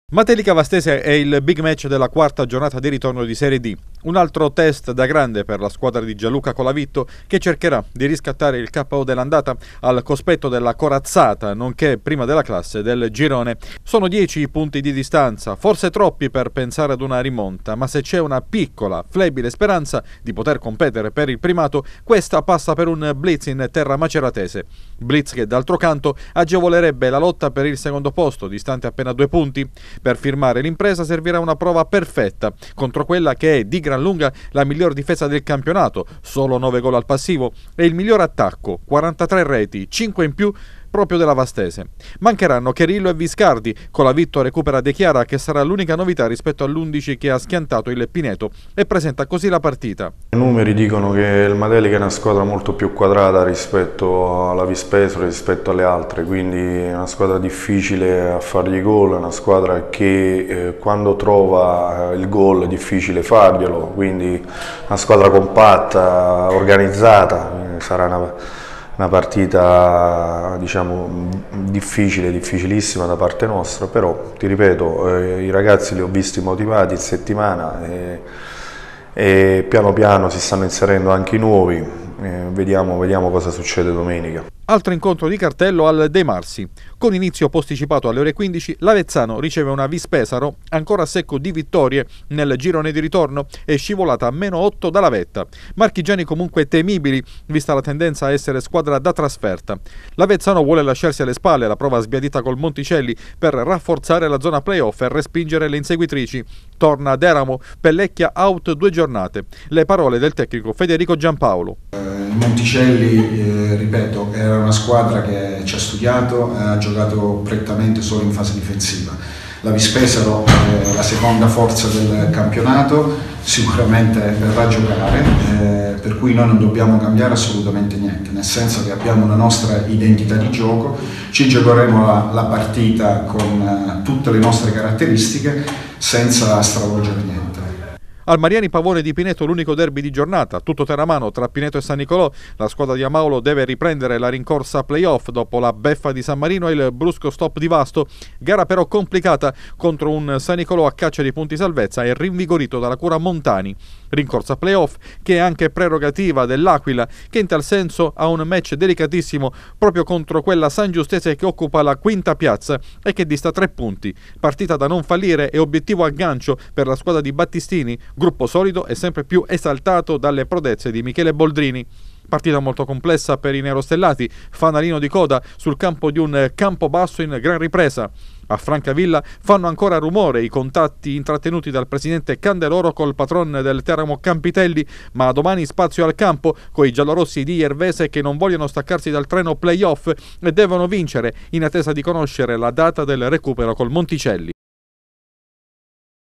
La Vastese a Matelica per riaprire il campionato
Dopo aver trovato la prima vittoria del 2018 la Vastese domani sarà di scena sul campo del Matelica, squadra dominatrice del campionato. Gare fondamentali in chiave playoff per Avezzano e Francavilla. A Pineto l’unico derby tra i padroni di casa e il San Nicolò. Nerostellati a Campobasso